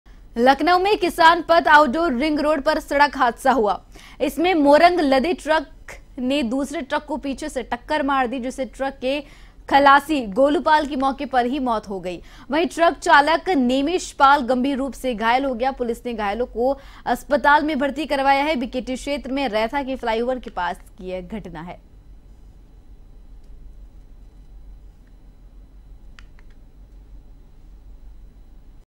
Lucknow: सड़क हादसे में खलासी की मौत, ट्रक चालक गंभीर रूप से घायल A1TV| Uttar Pradesh
सड़क हादसे में खलासी की मौत, ट्रक चालक गंभीर रूप से घायल

A1TV News Channel :

रविवार सुबह 8:30 बजे और शाम 4:30